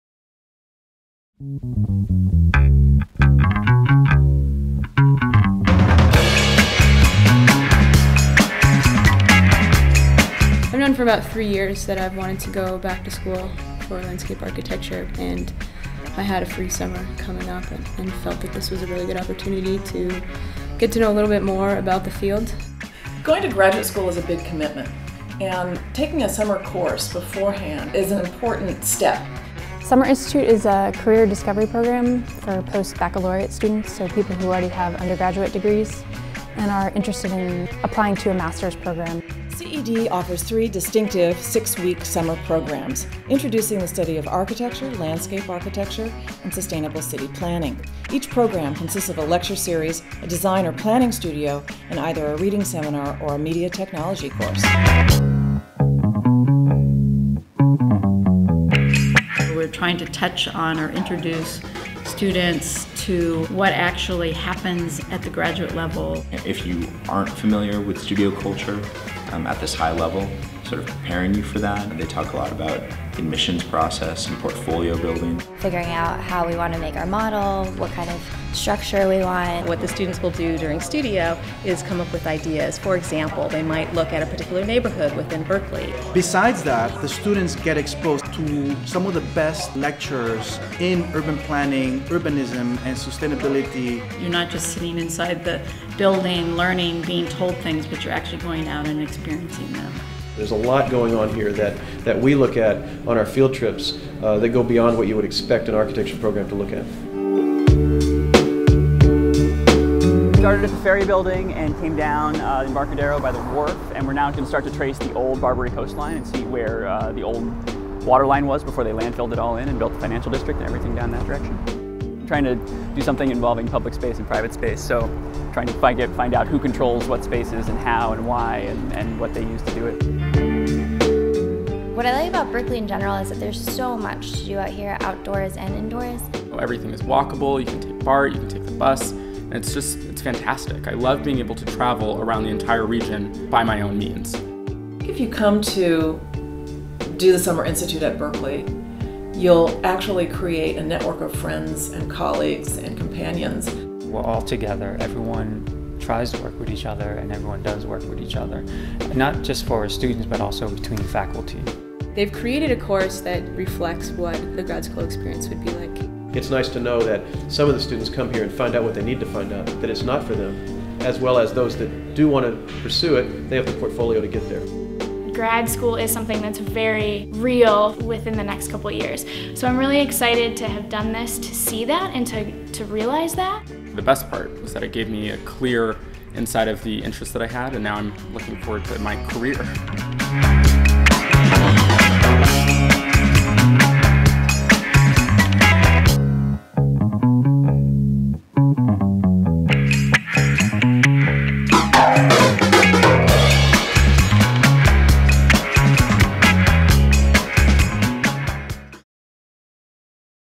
Summer Institute in Environmental Design, UC Berkeley
UC Berkeley's six-week summer programs in architecture, landscape architecture, and city planning let students who are interested in graduate study explore the methods and theories of the fields, experience the culture of design and planning studios, connect to top faculty and practitioners, and build a portfolio for graduate school application.